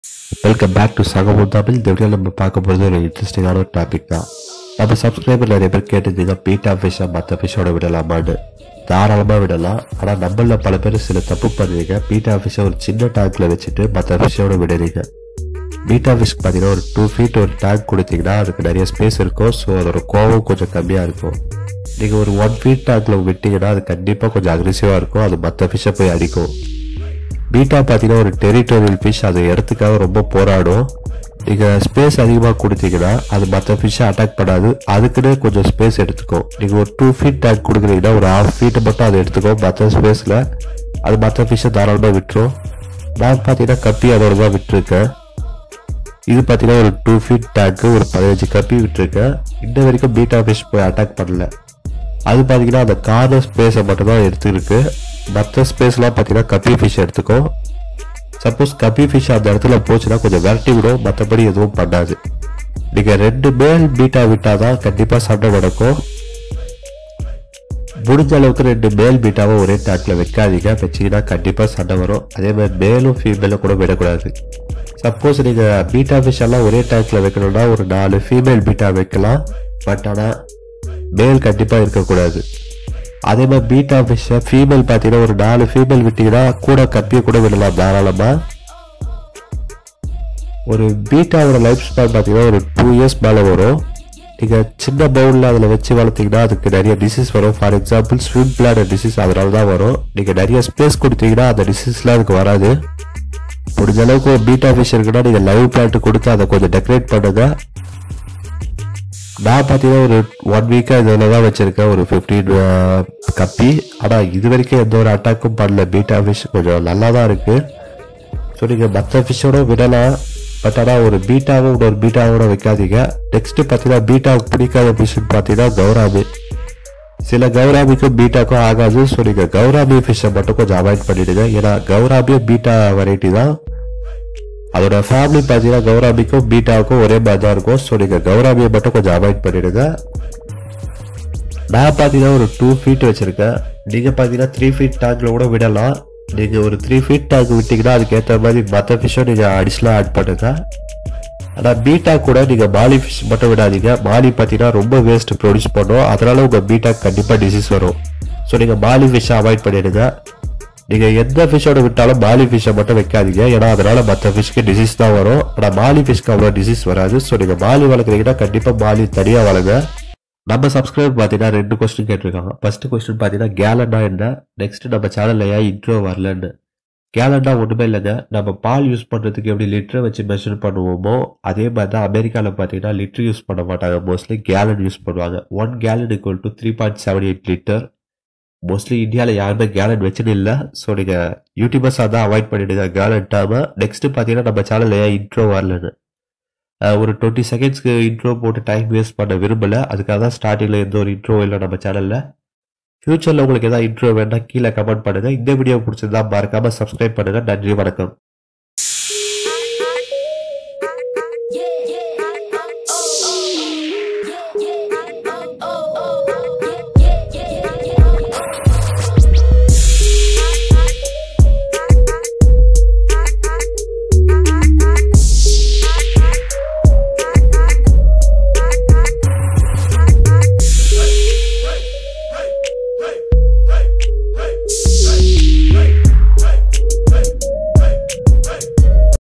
What happens when BETTA and Guppy are put together in a same tank
can betta and guppy share a tank,